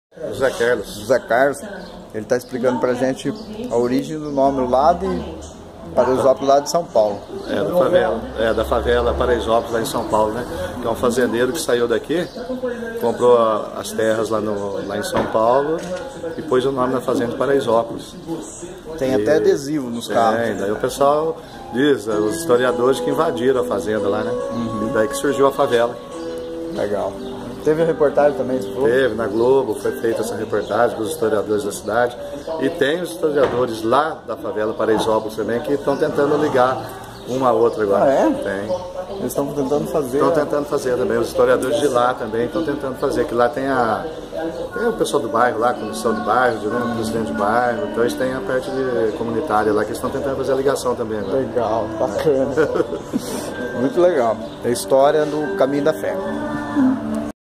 CaminhoDaFé 3º Dia - VD7 - Origem Paraisópolis SP
Caminho da Fé - 3º Dia saímos cedo, muito frio! A dificuldade continua  mas as paisagens lindas também